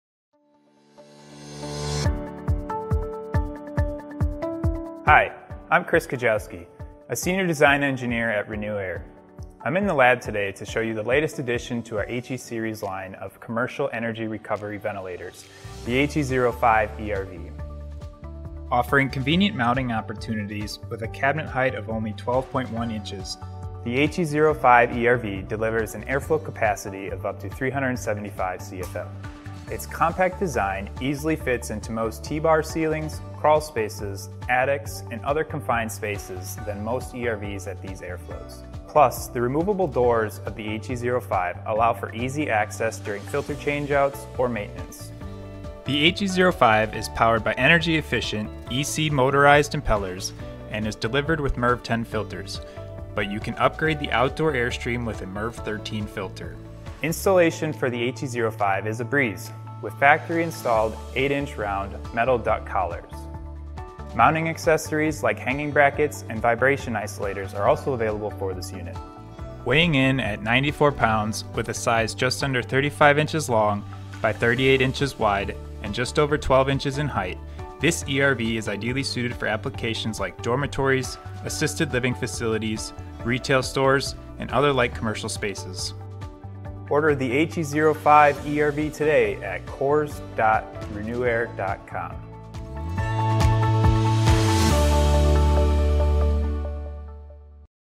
RenewAire HE05 ERV: Fits in T-Bar Ceilings (Up to 375 CFM) | Commercial Ventilation
**ULTRA-COMPACT COMMERCIAL ERV FOR T-BAR CEILINGS!** ⬇️ Introducing the RenewAire HE05 ERV, the latest addition to our H-Series line of commercial energy recovery ventilators. This unit is specifically designed to fit into confined spaces like T-Bar ceilings, crawl spaces, and attics, delivering an airflow capacity of up to 375 CFM.

### VIDEO CHAPTERS:

0:00 - Introduction to the HE05 ERV
0:11 - Convenient Mounting & Compact Cabinet Height (12.1 in.)
0:29 - Airflow Capacity (Up to 375 CFM) & Confined Space Fit
0:37 - Easy Maintenance (Filter change-outs)
0:44 - Energy-Efficient EC Motorized Impellers & Filter Options (MERV 10/13)
0:57 - Installation Features (8-in. collars) & Weight (94 lb)
1:16 - Ideal Applications (Dormitories, Retail, Assisted Living)
1:24 - How to Order the HE05

### HE05 ERV KEY FEATURES:

- **Ultra-Compact Design:** Cabinet height of only 12.1 inches fits easily into most T-Bar ceilings, attics, and crawl spaces.
- **High Performance:** Delivers up to 375 CFM.
- **Easy Maintenance:** Removable doors allow for easy filter change-outs and maintenance.
- **Efficient Operation:** Powered by energy-efficient EC motorized impellers.
- **Quick Installation:** Comes with factory-installed 8-inch round metal duct collars.